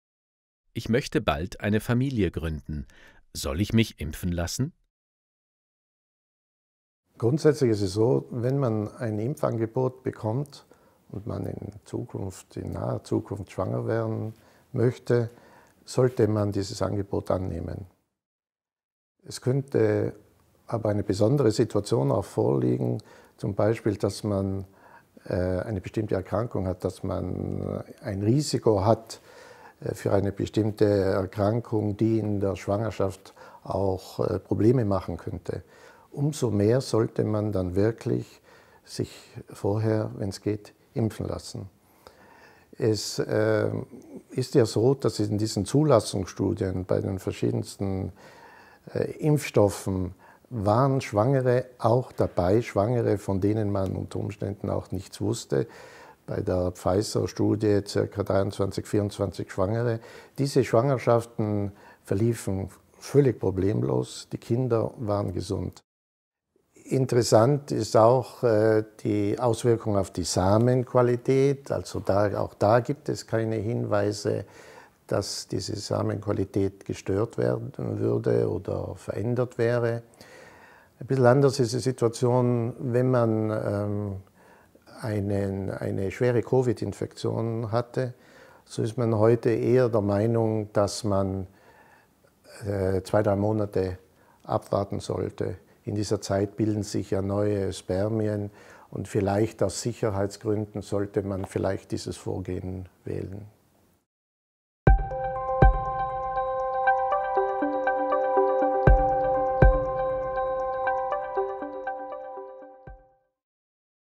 Ich möchte bald eine Familie gründen, soll ich mich impfen lassen?
Dr. Herbert Heidegger, Primar der Gynäkologie und Geburtshilfe am Krankenhaus Meran, klärt